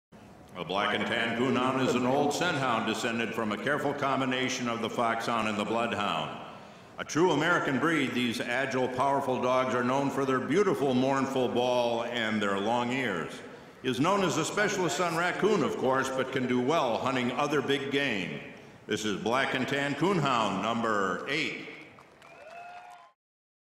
WKC Archives: Black and Tan Coonhound, Moses
Moses, GCH. Bayway Jersey's Due Process of Law, is the top winning male Black and Tan Coonhound in breed history. He is one of two Black and Tan Coonhounds to ever place in the Hound Group at the Westminster Kennel Club Dog Show since they made their first appearance in 1947.